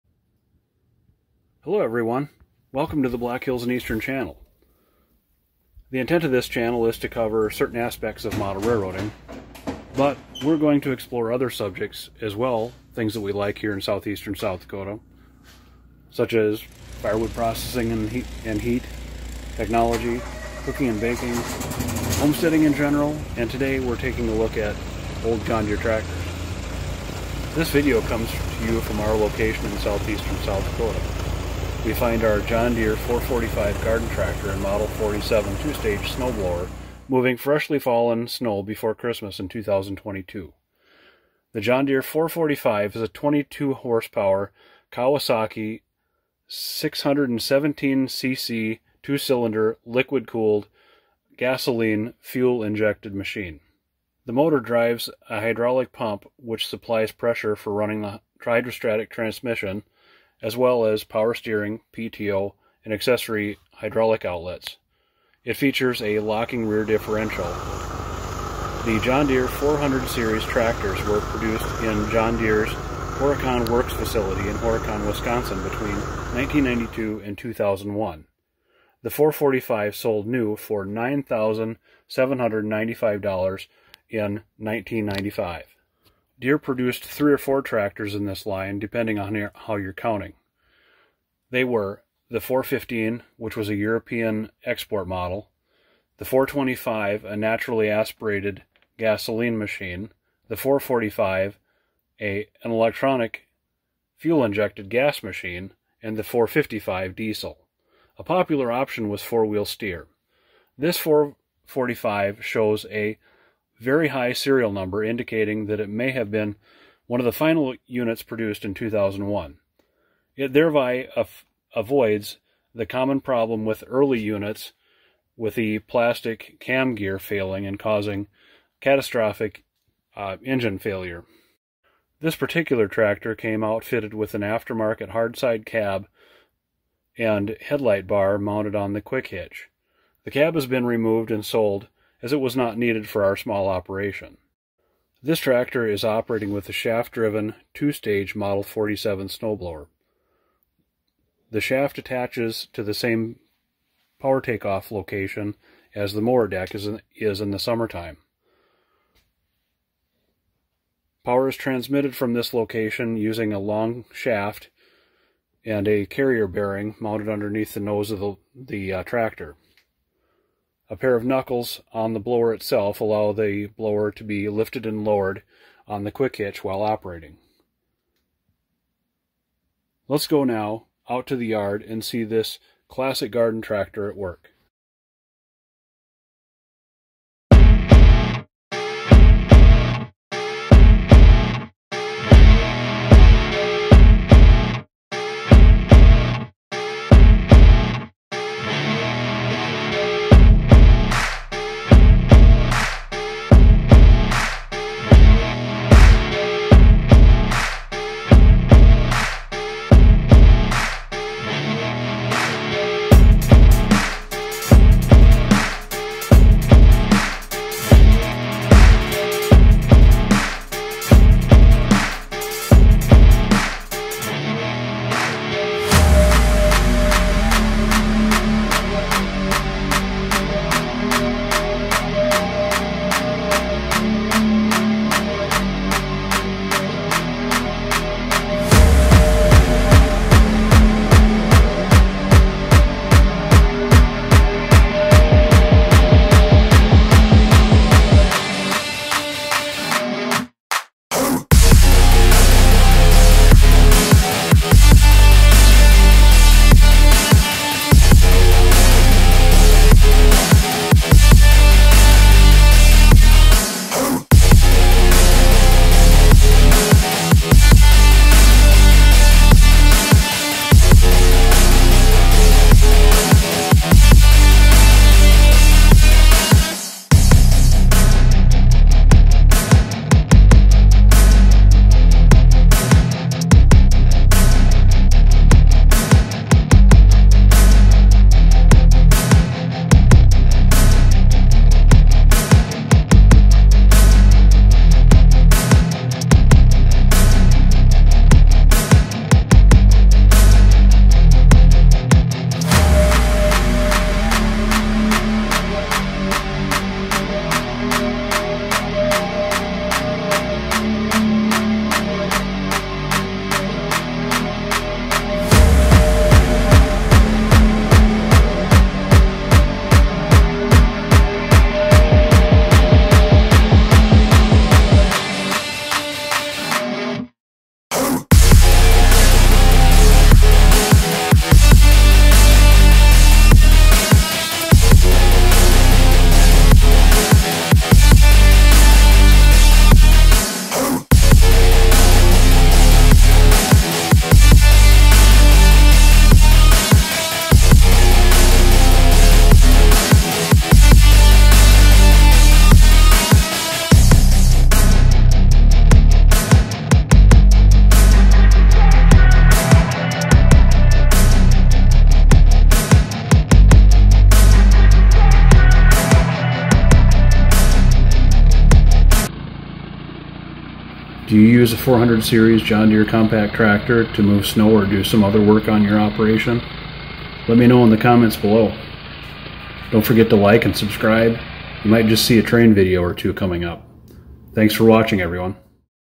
John Deere 445 Garden Tractor and 47 2 stage Snowblower at work in beautiful South Dakota scenery
Our John Deere 445 is a 22HP liquid cooled, fuel injected garden tractor built in Horicon, WI in 2001.  We've owned this tractor since 2014.

In December 2022, new snow fell, requiring the deployment of mechanized snow removal at our place and my dad's.  Enjoy the beautiful scenery as we work to clear the snow from our driveways.

Here's a video of a quick change of the JD 445 from mower to snowblower before a winter storm: